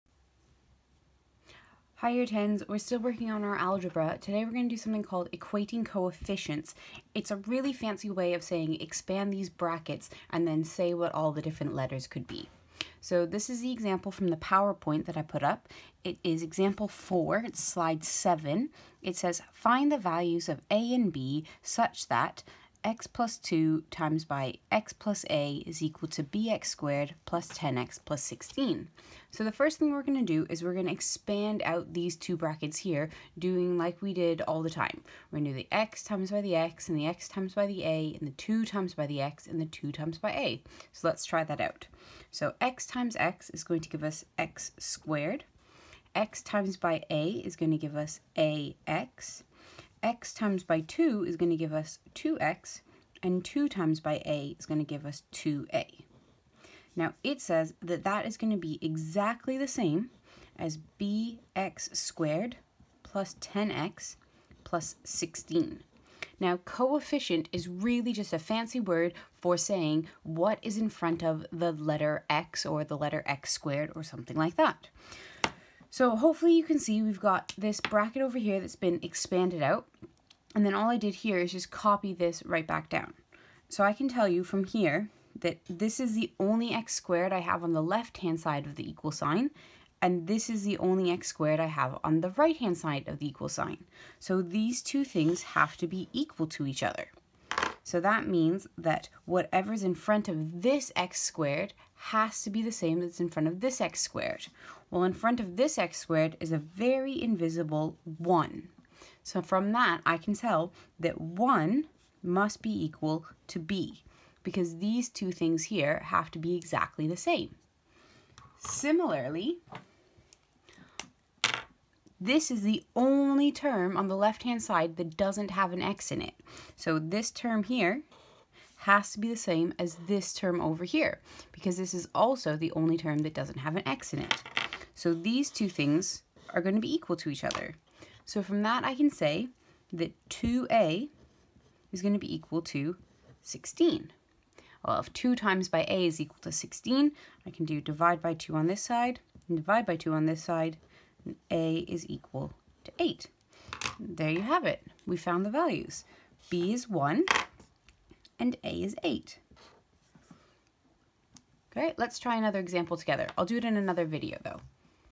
Equating Coefficients 1
New to the new GCSE, 'Equating Coefficients' to go with slide 7 of the powerpoint.